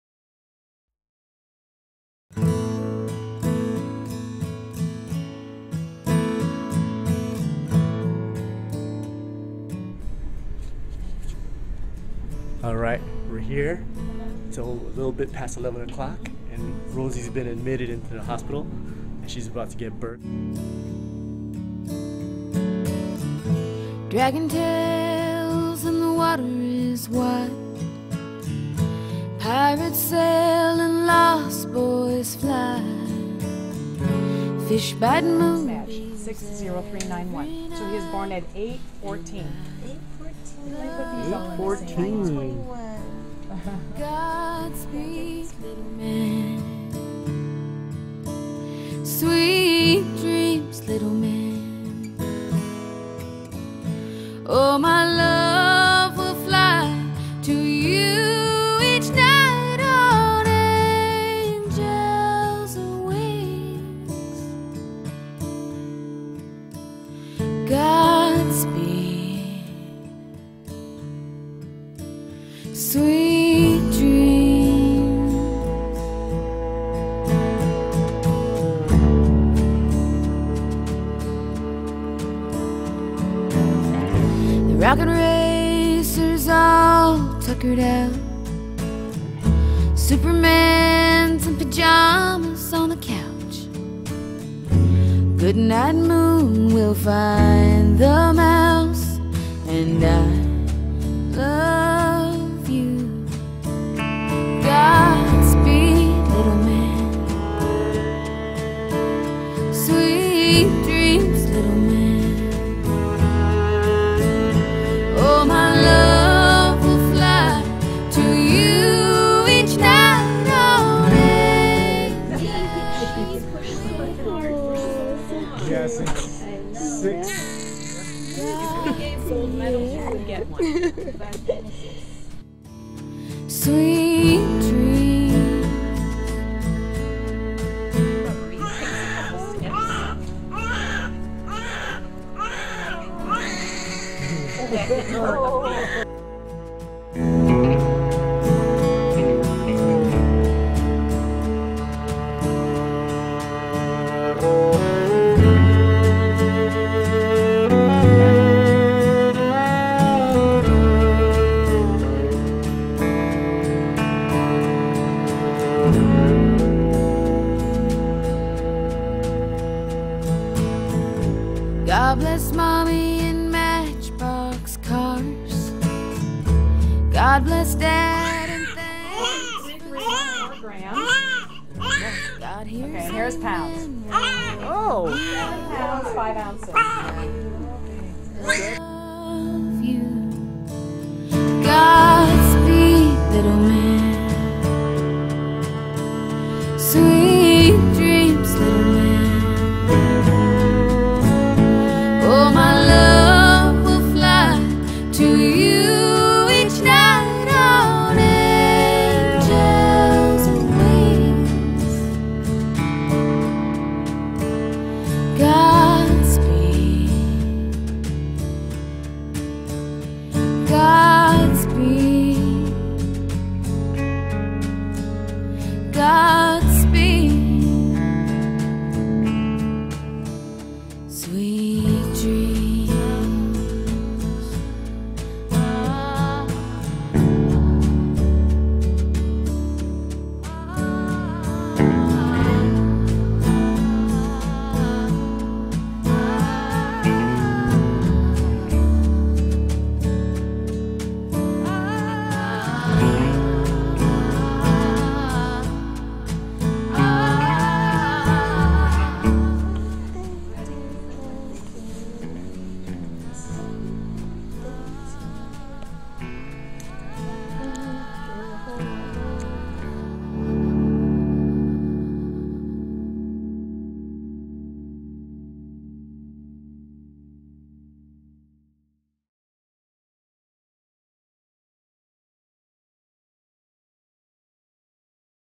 My First Born - Logan
Logan was born September 21, 2010 at 8:14AM PST. Weighing in at 7.49 pounds and 21 something inches.  We welcome you to our lives!